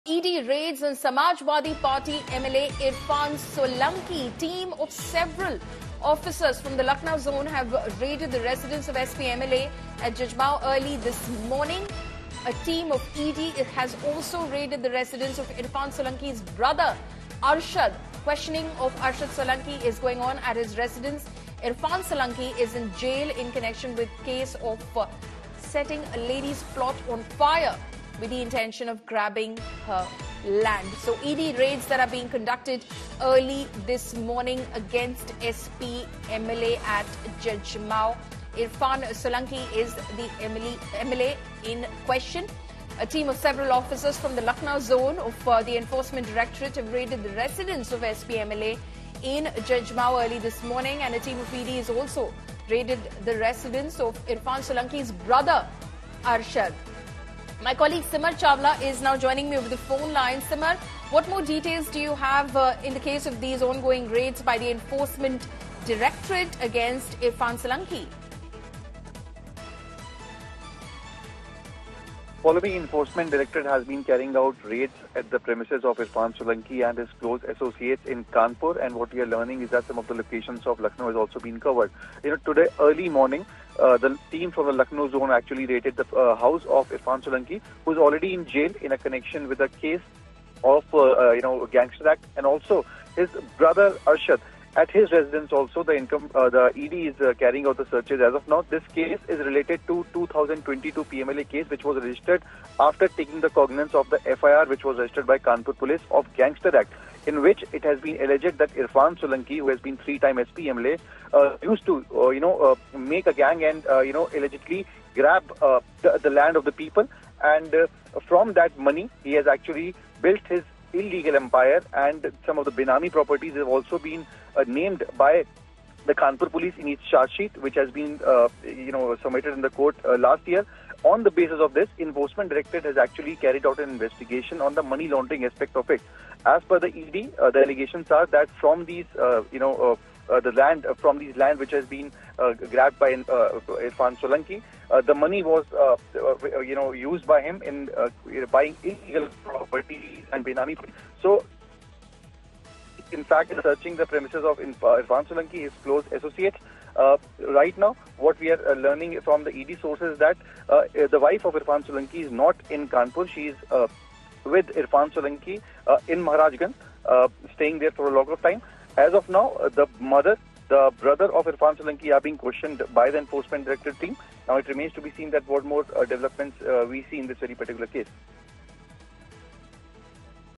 E.D. Raids Samajwadi Party MLA Irfan Solanki's Residence in Jajmao
In a surprise move, the Enforcement Directorate conducted early morning raids on the residence of Samajwadi Party MLA Irfan Solanki in Jajmao. The team of officers from the Lucknow zone also raided the residence of Solanki's brother, Arshad. The questioning of Arshad Solanki is ongoing at his residence. Irfan Solanki is currently in jail in connection with a case of setting a lady's plot on fire with the intention of grabbing her land.